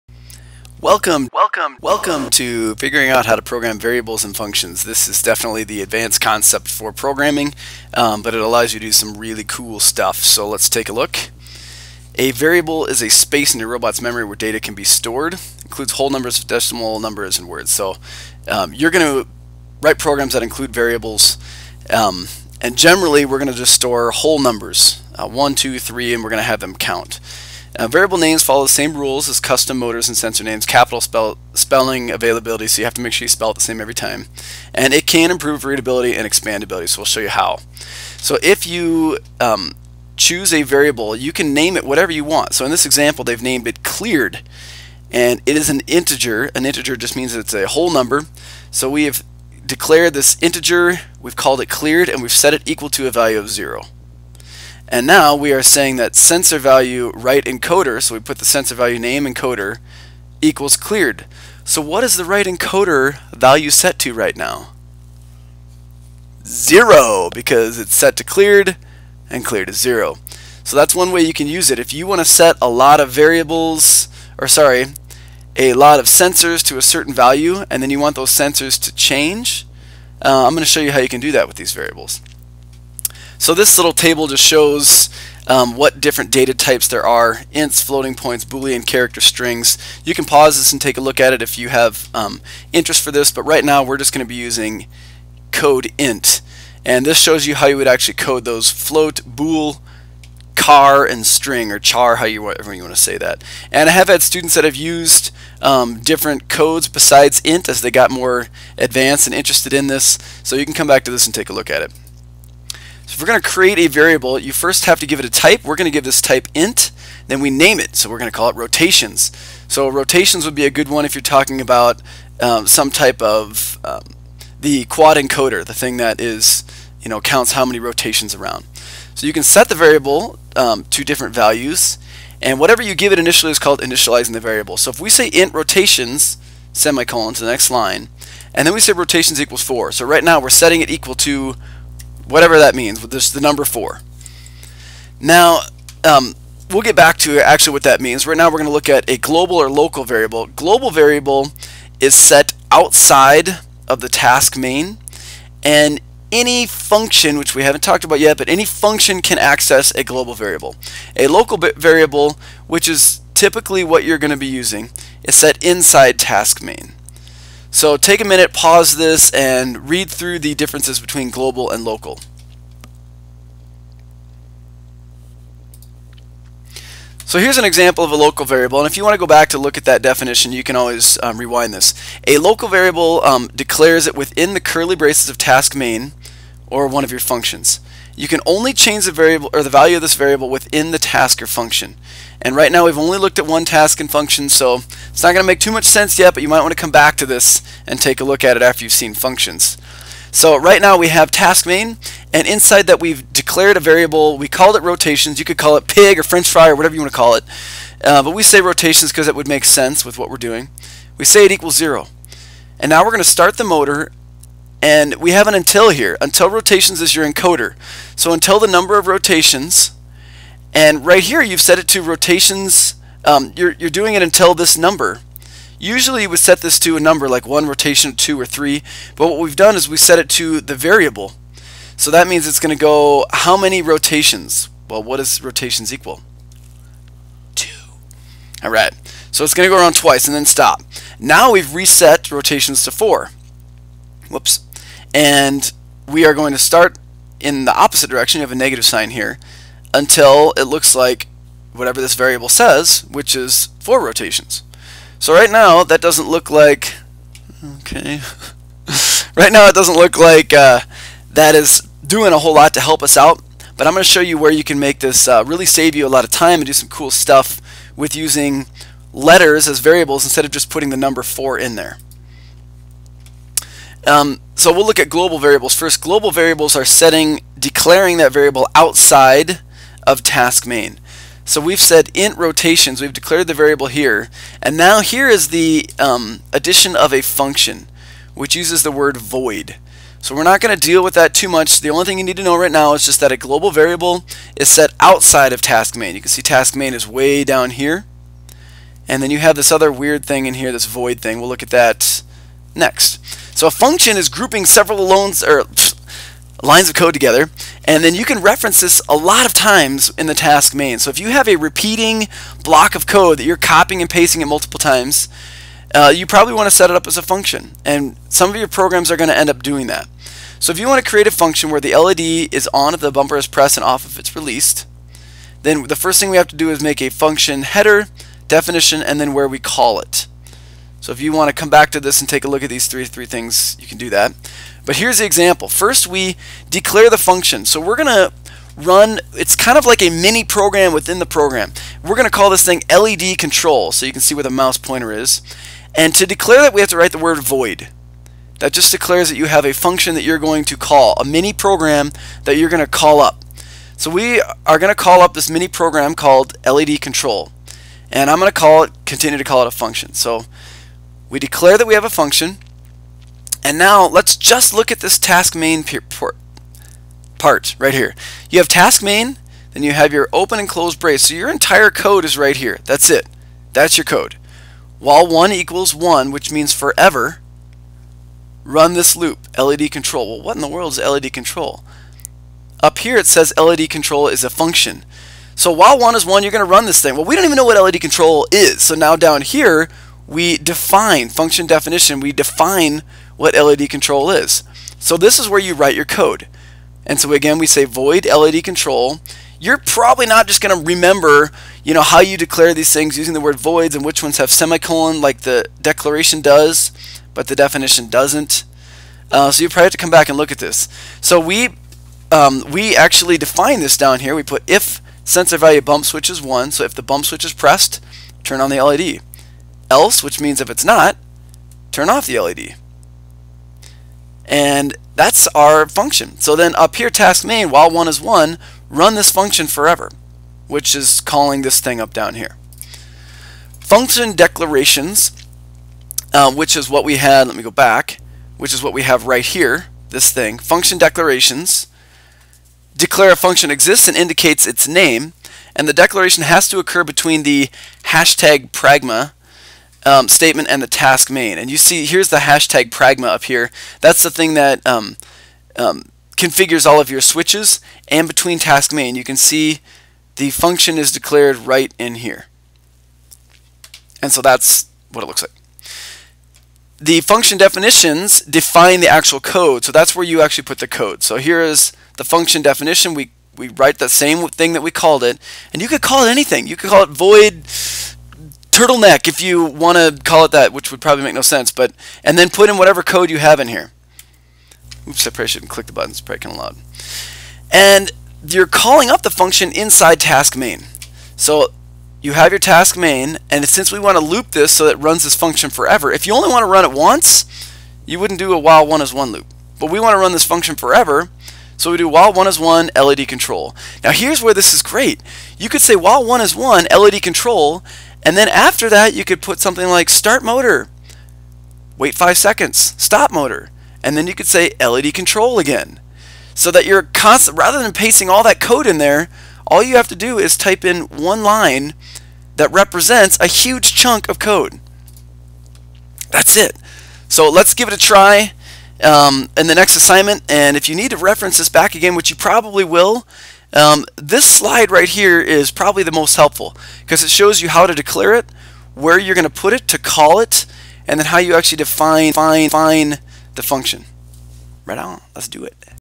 Variables & Functions
How to program with variables and functions using RobotC